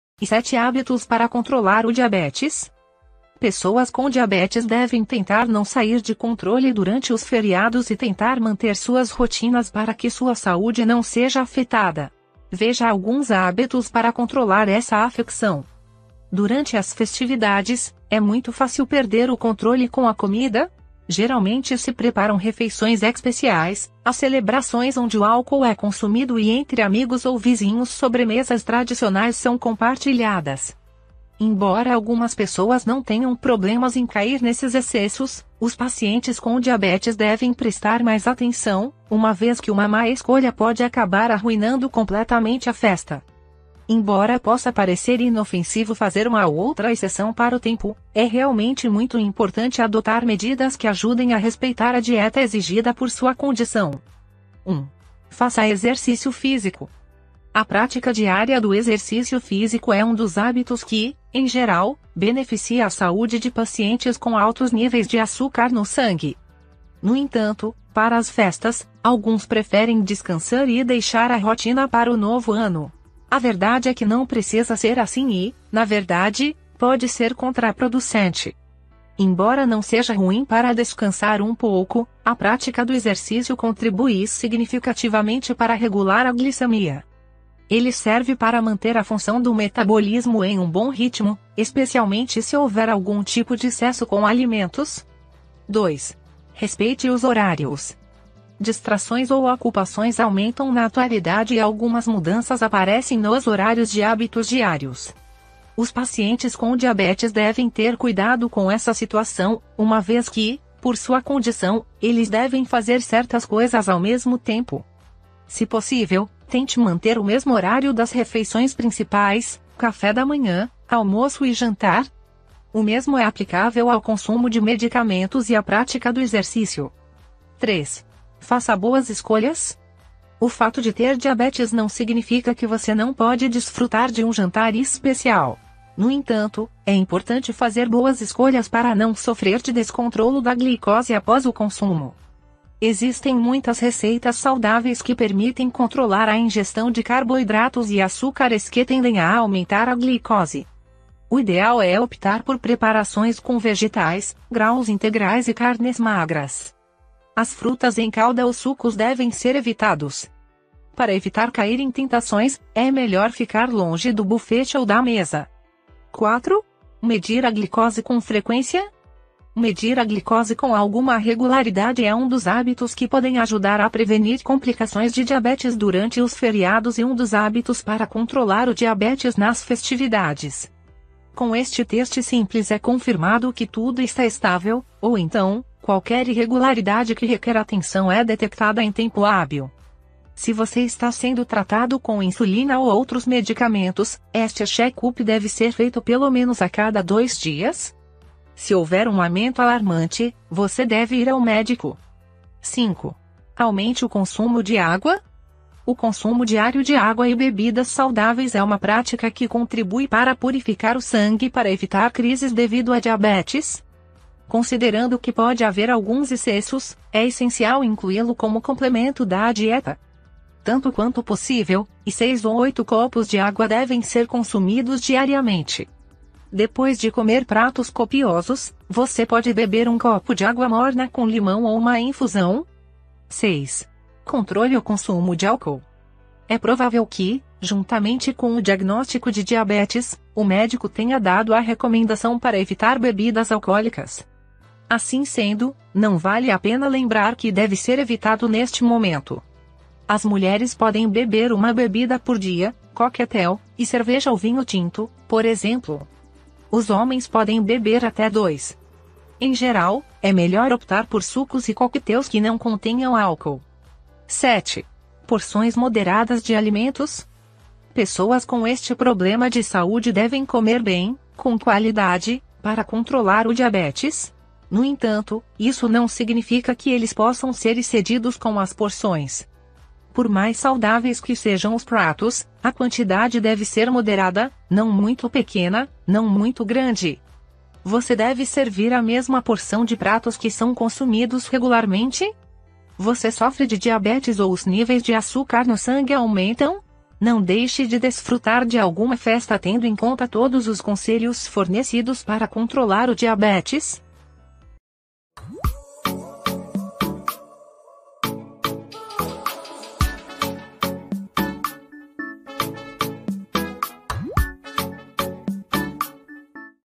Conheça 7 Hábitos Saudáveis em Festas e Feriados para Controlar o Diabetes com Saúde
7 Hábitos Saudáveis em Festas e Feriados par Controlar o Diabetes
Pessoas com diabetes devem tentar não sair de controle durante os feriados e tentar manter suas rotinas para que sua saúde não seja afetada. Veja alguns hábitos para controlar essa afecção.

Durante as festividades, é muito fácil perder o controle com a comida. Geralmente se preparam refeições especiais, há celebrações onde o álcool é consumido e entre amigos ou vizinhos sobremesas tradicionais são compartilhadas.

INDECI DO VÍDEO:

0.0 4: INSCREVA SE :)
0.0 4:  Conheça 7 Hábitos Saudáveis em Festas e Feriados para Controlar o Diabetes com Saúde